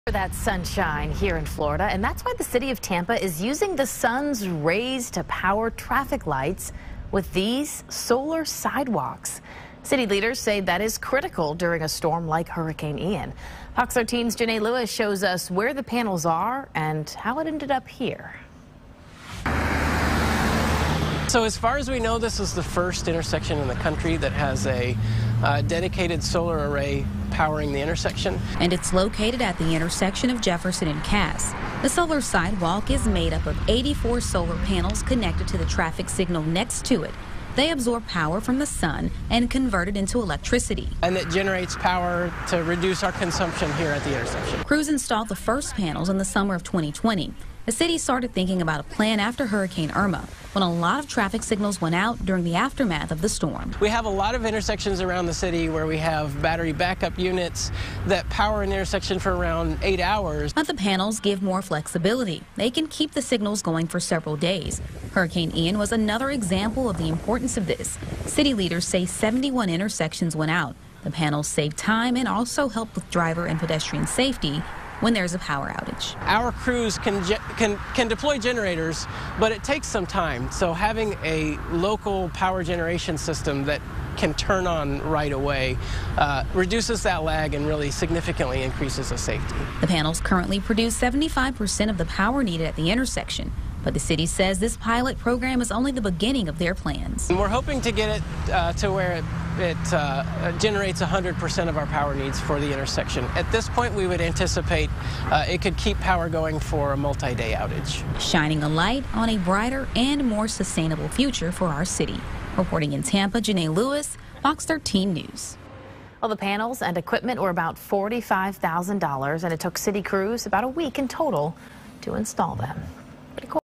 Tampa testing solar power sidewalks to power traffic lights
The city of Tampa is experimenting with solar sidewalks to see if they can power nearby traffic lights, which would help the flow of traffic when there is no electricity after a hurricane.